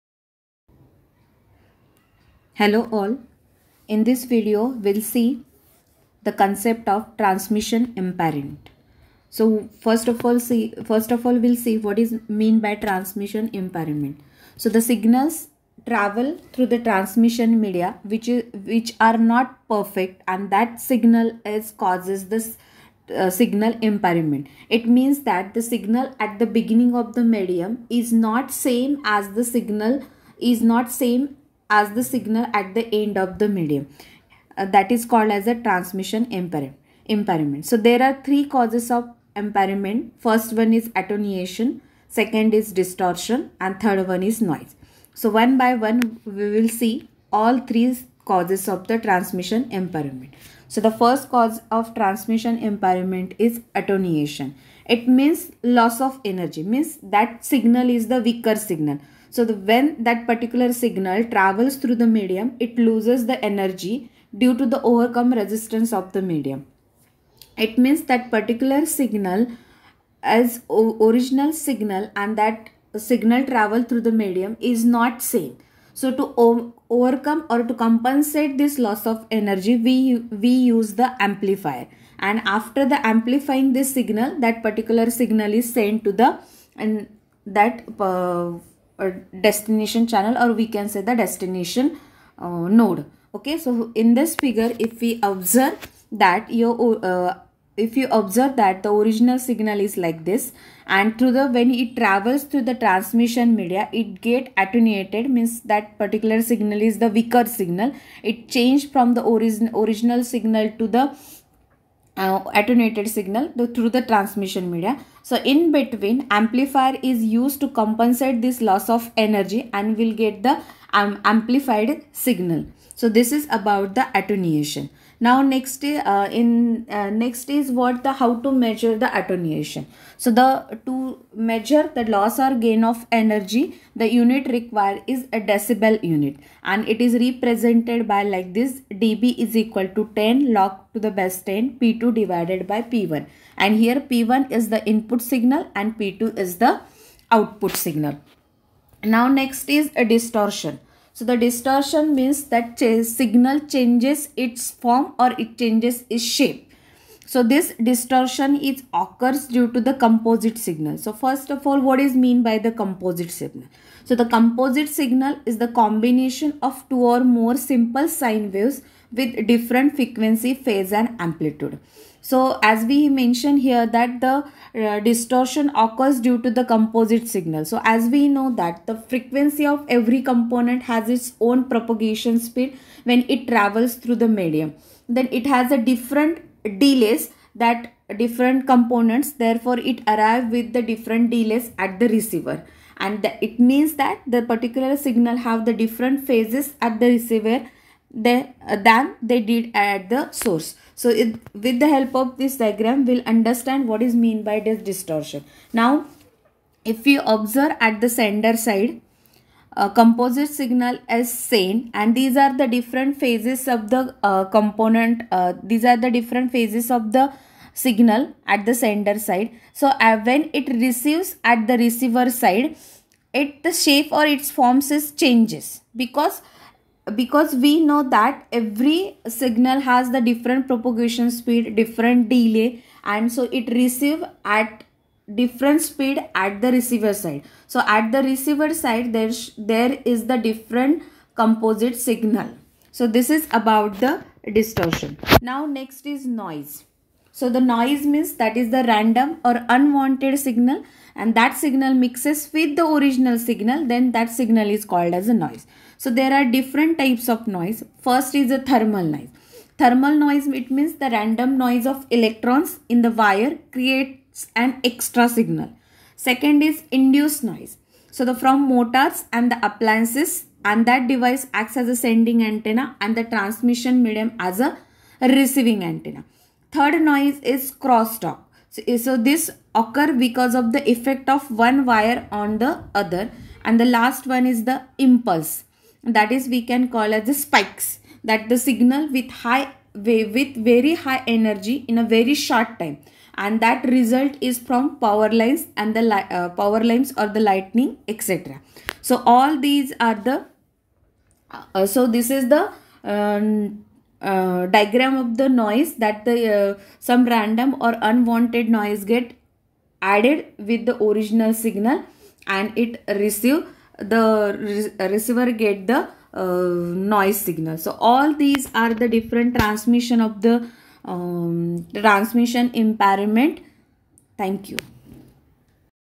Transmission imp
Subject : Computer Networks
Video by- Mrs.M.V.Shelke
Title of the video – Transmission Impairment
Description - Signals travel through transmission media, which are not perfect. The imperfection causes signal impairment. Three causes of impairment are attenuation, distortion, and noise.